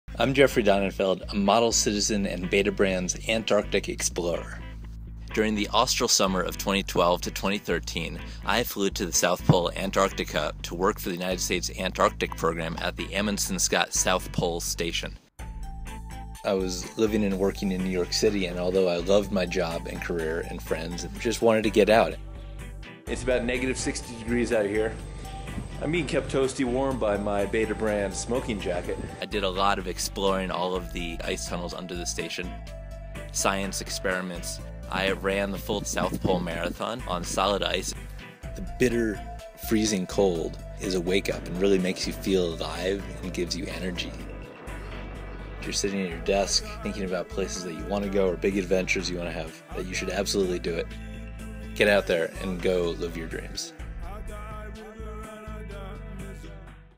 Betabrand's Antarctic Explorer
-- Watch Model Citizen Jeffrey Donenfeld explore the South Pole as part of the U.S. Antarctic Program at the Amundsen-Scott Station.